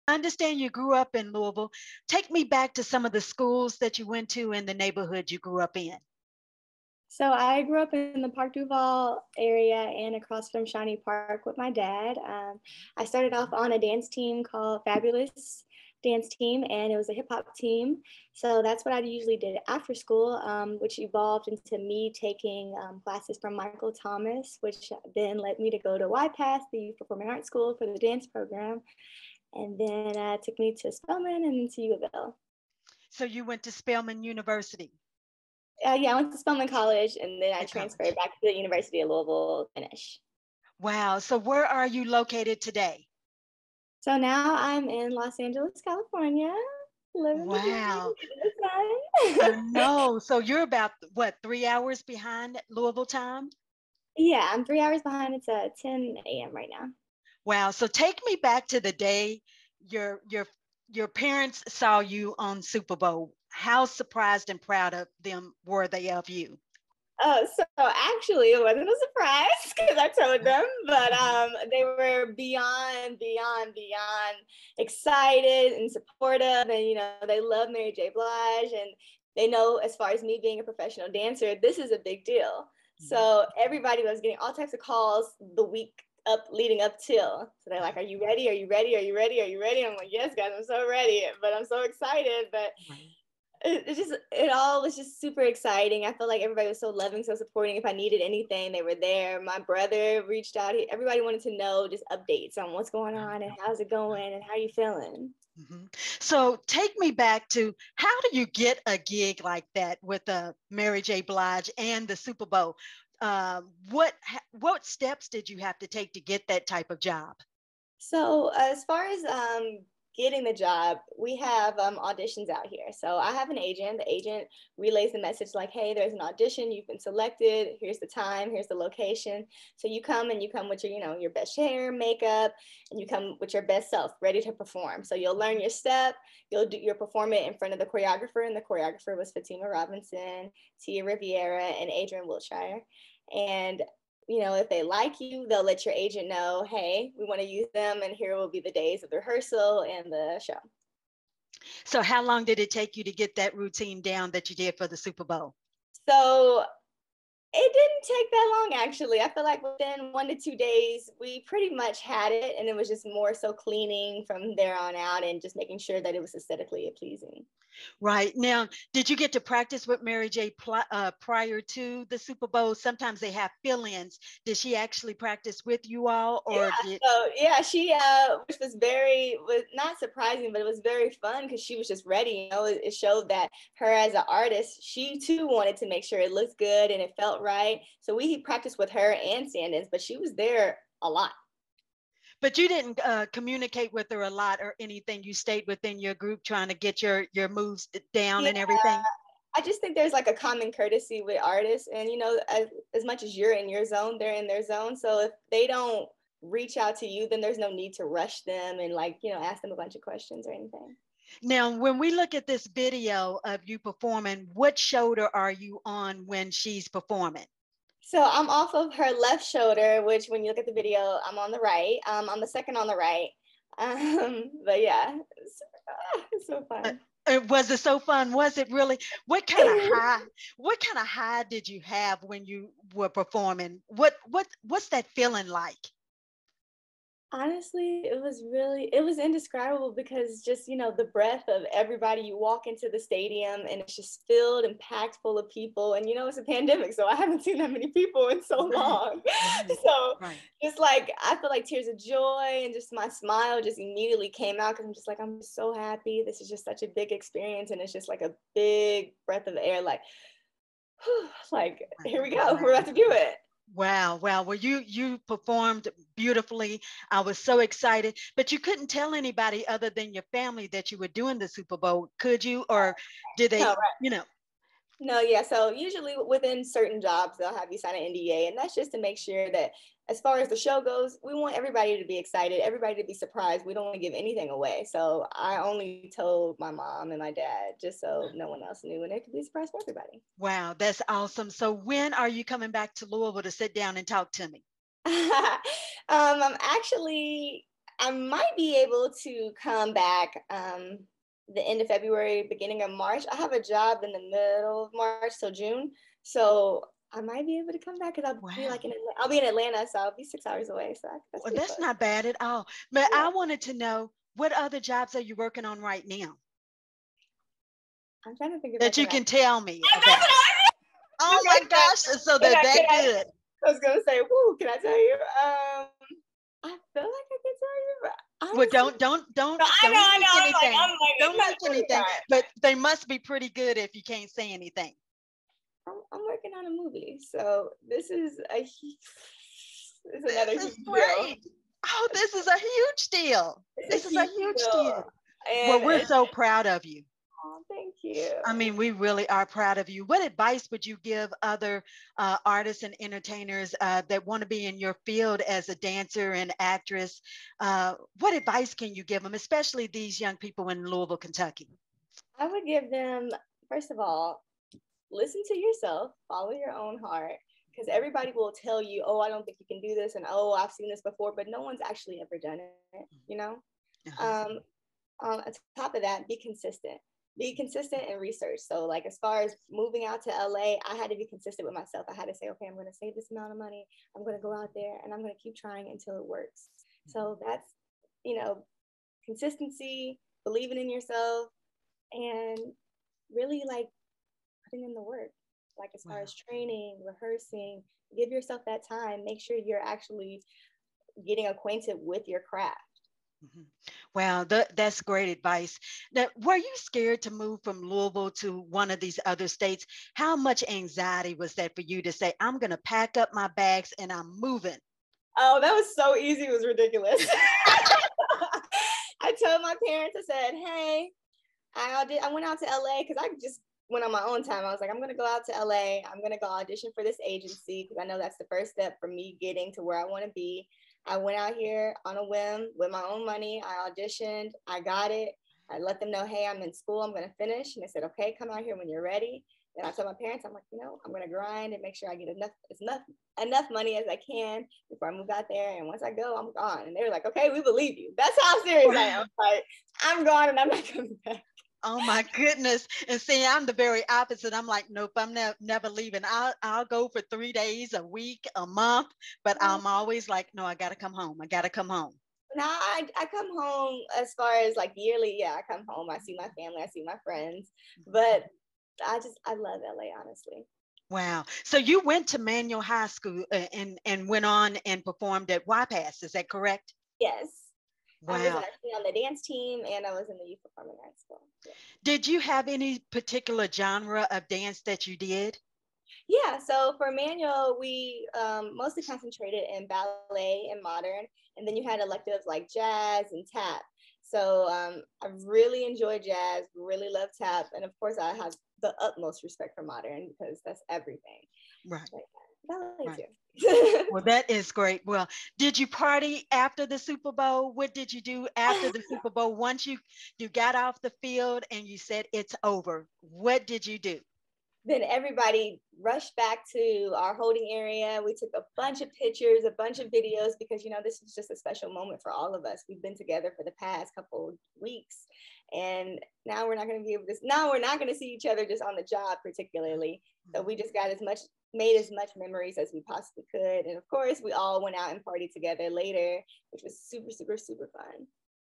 Full interview with Jhana Waddell, a Louisville native who performed at Super Bowl LVI halftime show
Jhana Waddell's no stranger to big performances. She's danced alongside Selena Gomez and Bruno Mars and has even toured with Lil Baby and Future.